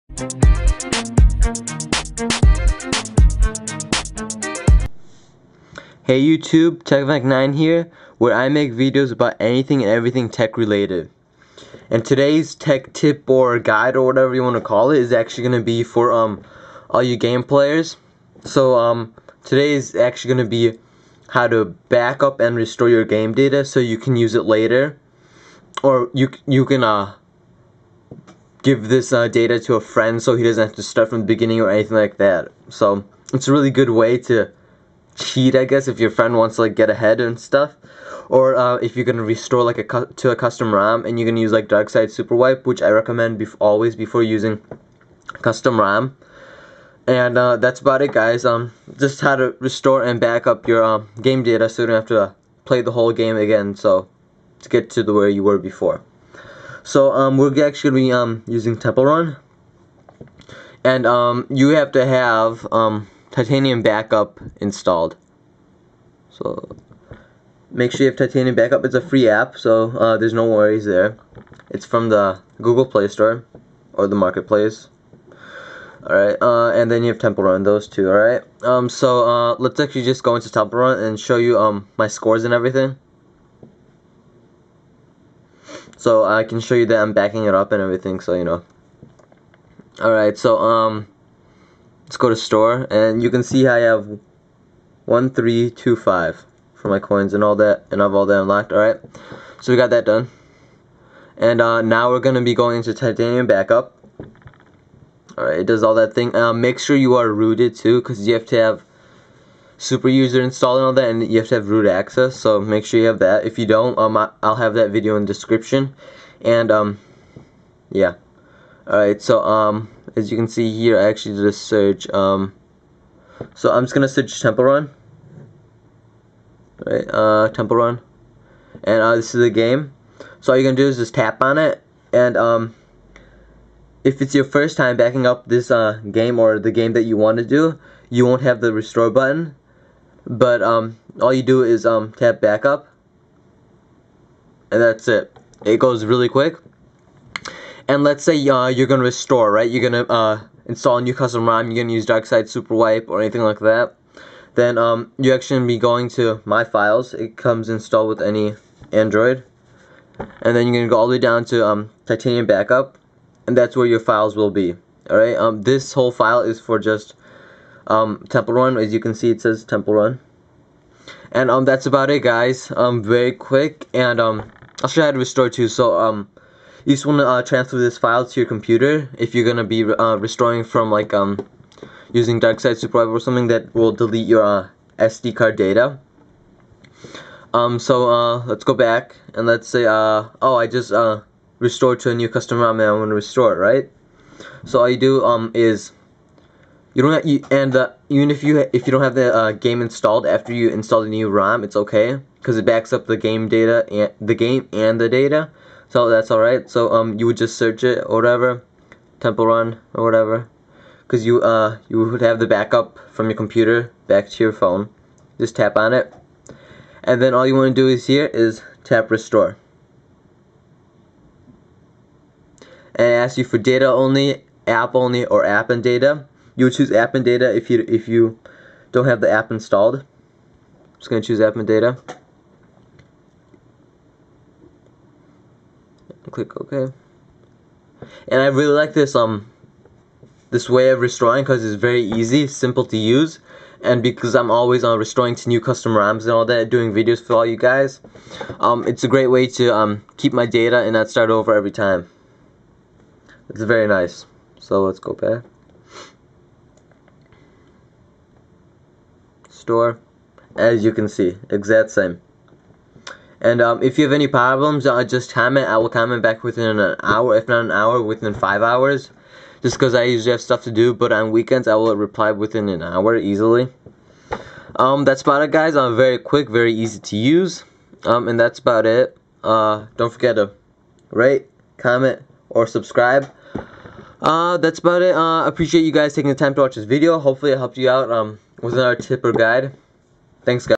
How To Back and Restore Game Data On Any Android Device
Today I'm going to show you how to backup your existing game data ( from any app) and restore to your phone or to another person's phone.

This process will work with any type of Android device

Note: You must be rooted, if your not here is the link on how to root your Samsung Galaxy S2 phone

After you have used Titanium Backup ( root access needed) you can transfer the file to your desktop and then restore to another custom rom or restore you phone.

Then just move the file back to your phone then go back into Titanium Backup and restore the app. It will restore the game and data.

This process is great if you hate losing game data.
I use it all the time and I love it!